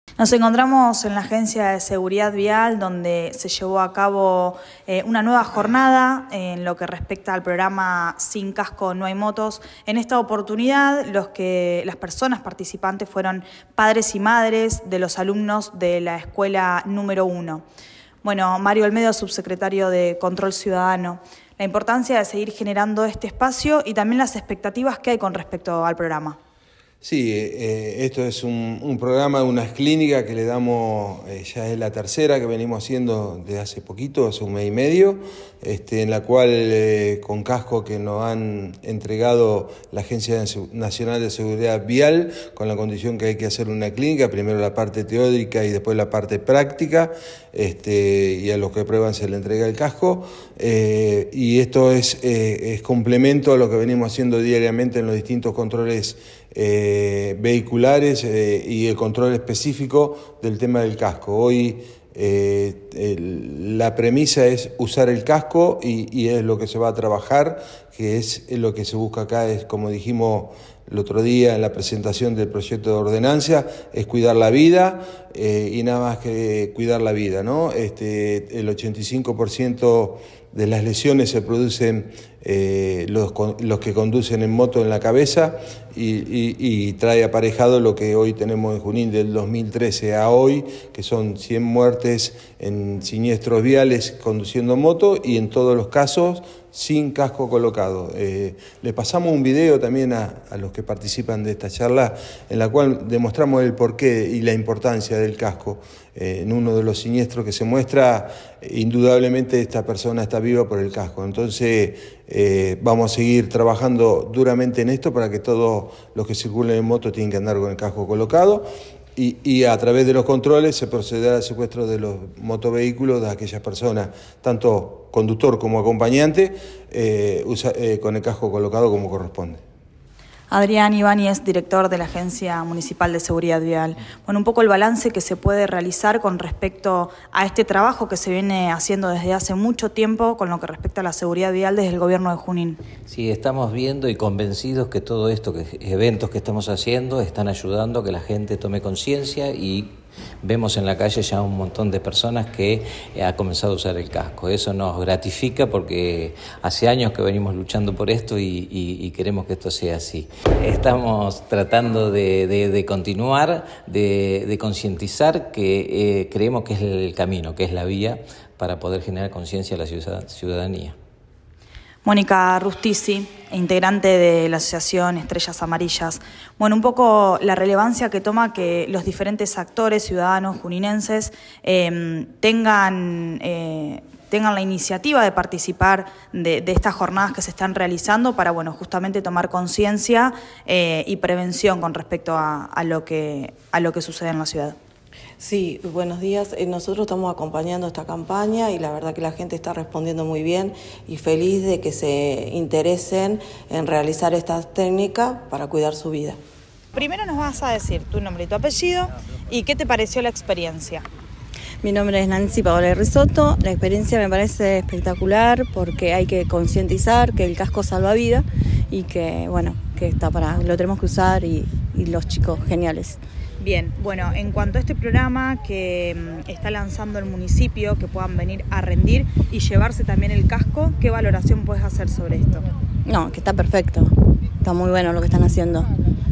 Continúan con las capacitaciones sobre manejo responsable del programa “Sin casco no hay moto”
Con el propósito de seguir fomentando el respeto por las reglas de tránsito y concientizar sobre el uso del casco para reducir accidentes, la Agencia Municipal De Seguridad Vial del Gobierno de Junín brindó un nuevo curso sobre manejo seguro de moto y seguridad vial destinado a las familias de los alumnos y alumnas que asisten a la Escuela N° 1. La jornada estuvo integrada en un principio por una parte teórica y luego, los participantes recorrieron el circuito de práctica, al finalizar recibieron un casco.

Desde el Municipio destacaron que estos cursos se van a continuar planificando, y en este lineamiento se seguirán coordinando acciones con distintos actores de la ciudad. Además, desde la Asociación Civil Estrellas Amarillas valoraron el compromiso por parte del Municipio en generar iniciativas para prevenir lesiones y muertes en siniestros viales, promoviendo la concientización en la comunidad.